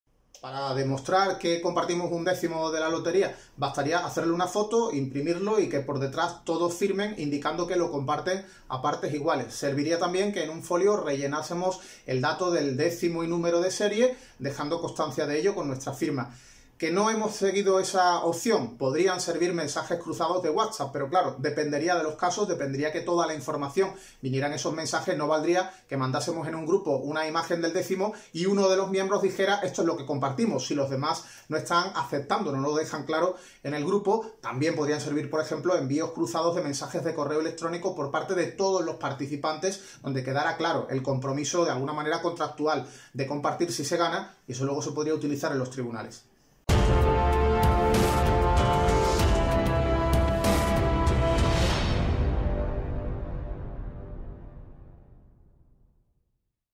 Cómo demostrar que compartías un décimo de Lotería premiado
El secretario general de FACUA, Rubén Sánchez, explica cómo demostrar que se compartía un décimo de lotería premiado en el sorteo de la Lotería de Navidad para este martes 22 de diciembre.